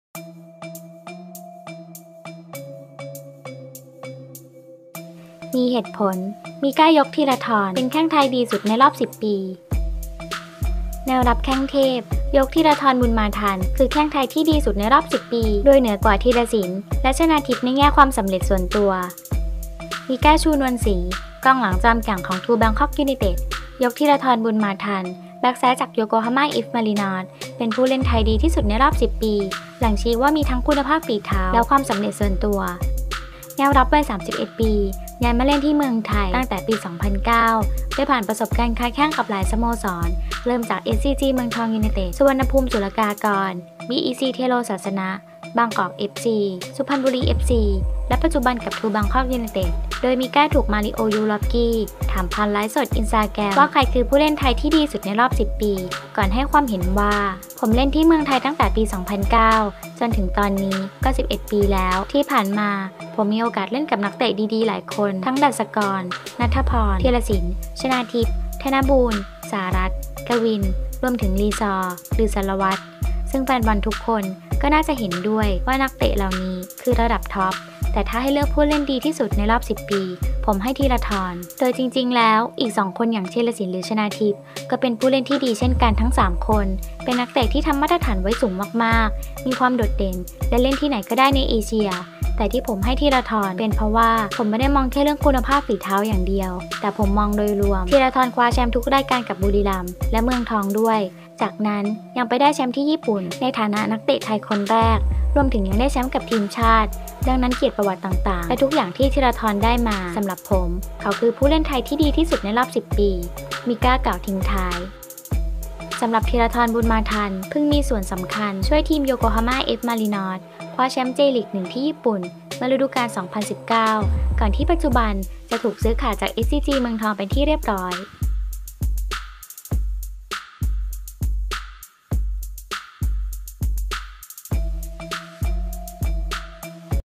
มิก้ายก "ธีราทร"แข้งไทยดีสุดรอบ10ปี
มีเหตุผล!มิก้ายก"ธีราทร"แข้งไทยดีสุดรอบ10ปี

แนวรับแข้งเทพ ยก ธีราทร คือแข้งไทยดีสุดรอบ 10 ปี โดยเหนือกว่า ธีรศิลป์ และ ชนาธิป ในแง่ความสำเร็จส่วนตัว